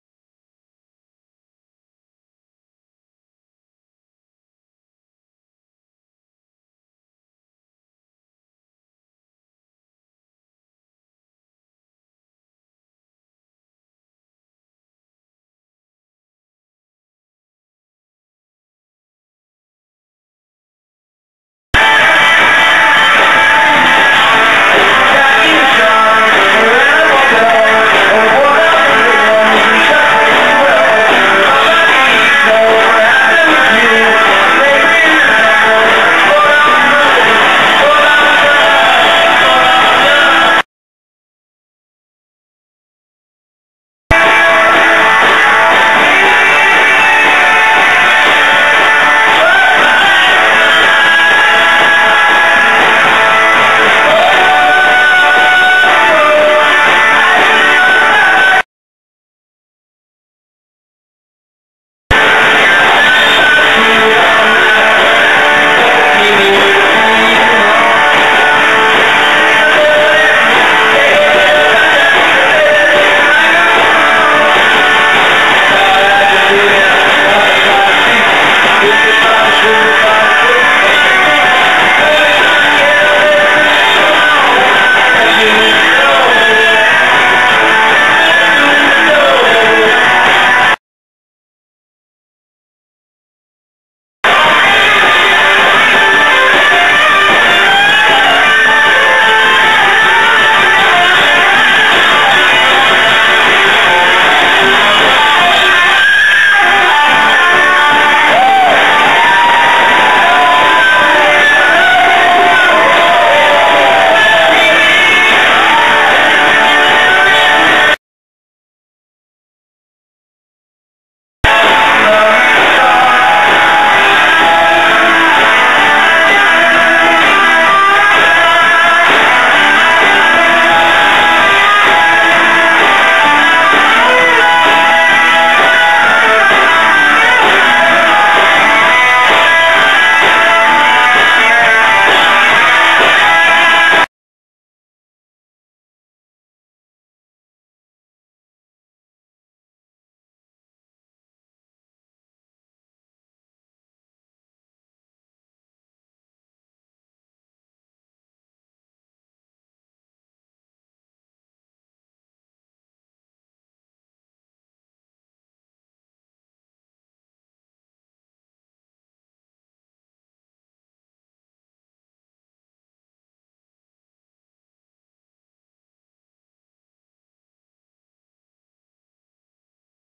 Northampton Band Treefort Live Clips from Brass Cat 2004
Northampton band Treefort at The Brass Cat in Easthampton MA. Very short clips, shot on a still camera in video mode. I'm filling in on guitar. Video is a little rough-and-ready, but this is part of the band's record, so up it goes. More Treefort video, with the full lineup, can be found on Facebook. (Dig that Fenway Green Monster wall near the stage...)

Brass Cat nightclub, Downtown Easthampton, Ware River Club, Matt Hebert, Fancy Trash, Figments